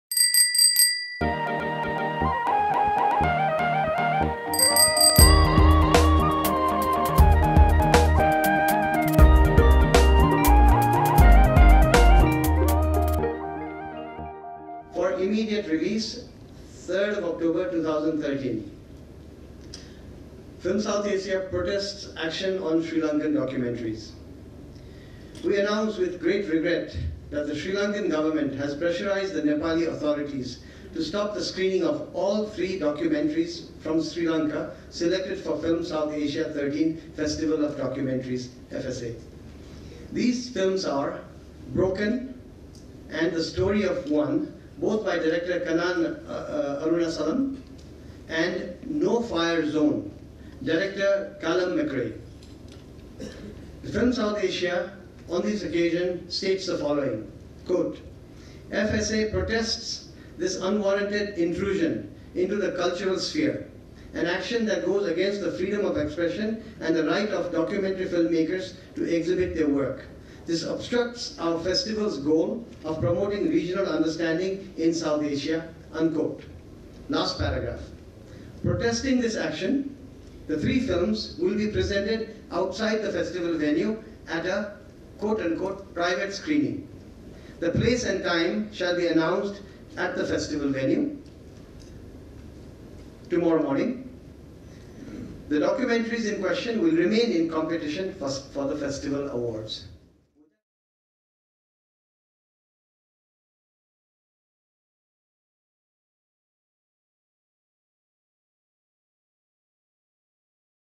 Sri Lankan Films censored on FSA
PRESS RELEASE

Film Southasia 2013

3rd October 2013
Film Southasia protests action on Sri Lankan documentaries

We announce with great regret that the Sri Lankan Government has pressurised the Nepali authorities to stop the screening of all three documentaries from Sri Lanka selected for the Film Southasia '13 festival of documentaries (FSA).

These films are: 'Broken' and 'The Story of One' (both by director Kannan Arunasalam), and 'No Fire Zone' (director Callum Macrae).

Kanak Mani Dixit, Chair of Film Southasia, said: "FSA protests this unwarranted intrusion into the cultural sphere, an action that goes against the freedom of expression and the right of documentary filmmakers to exhibit their work. This obstructs our festival's goal of promoting regional understanding in Southasia."

Protesting this action, the three films will be presented outside the festival venue at a 'private screening'. The place and time shall be announced at the festival venue. The documentaries in question will remain in competition for the festival awards.

FSA2013 is the Official Hash Tag of Film Southasia 2013.